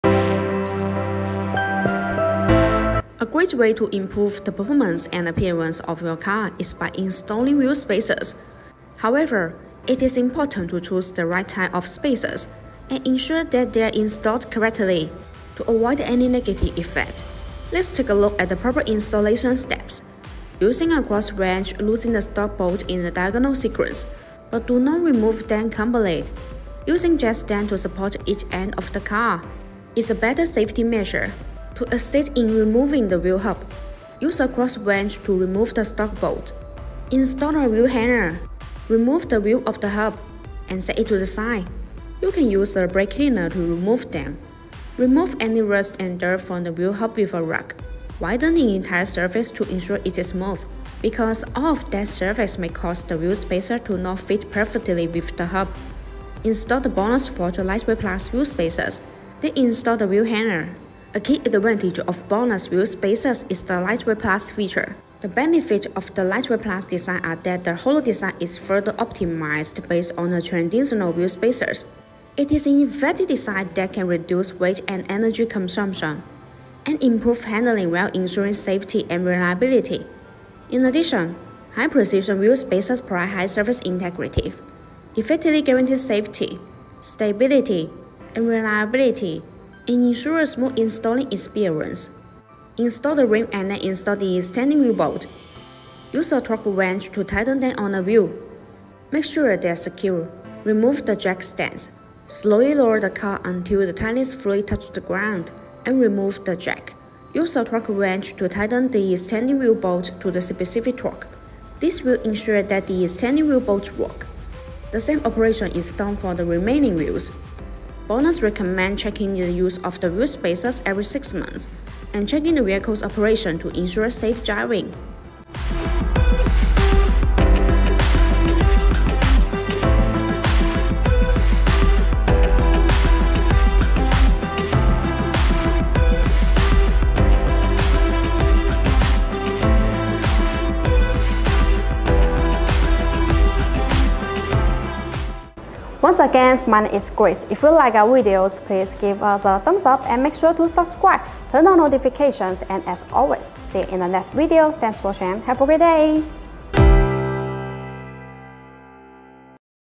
How To Install BMW 2 Series G42 Wheel Spacers? Step-by-Step Guide! - BONOSS BMW Parts
If this is your first time fitting wheel spacers to your BMW 2 Series G42, this video will help.
BMW G42 wheel spacers are devices that fit between the wheel hub and the wheel, creating a gap that pushes the wheel outward. This can reduce the wheel's offset, which is the distance between the wheel's centerline and the hub's mounting surface. A lower offset can increase your BMW 2 Series G42's track width, which is the distance between the centerlines of your left and right wheels. This can improve the stability, cornering, and appearance of your BMW 2 Series G42.

Installing wheel spacers on your BMW 2 Series G42 is not a simple plug-and-play process. You need to make sure that you choose the right size and type of wheel spacers for your car, and that you install them properly and securely. Otherwise, you risk damaging your wheels, tires, suspension, brakes or even causing an accident.

In this video, we will show you step-by-step how to fit wheel spacers on your BMW 2 Series G42 correctly and safely. Before we begin, make sure you have the following tools and materials.

A jack and jack stands
A wheel lug wrench and torque wrench
An impact wrench
A wire brush or sandpaper
A set of hub-centric wheel spacers that match your BMW 2 Series G42's bolt pattern, hub bore, and desired thickness.

🔜🛒Click Here for the BONOSS Forged Lightweight Plus BMW Wheel Spacers for your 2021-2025 BMW 2 Series G42 218i/ 220i/ 230i/ M240i.

📄Information:
Vehicle: BMW 2 Series G42 218i/ 220i/ 230i/ M240i
Product: BONOSS Forged Lightweight Plus Wheel Spacers & BONOSS Forged Grade 12.9 Extended Wheel Bolts (formerly bloxsport)
Material: Aluminum Alloy 6061-T6/7075-T6(Spacers)/ SCM440(Bolts)
All Thickness: 3mm-20mm
Customized: 3mm-160mm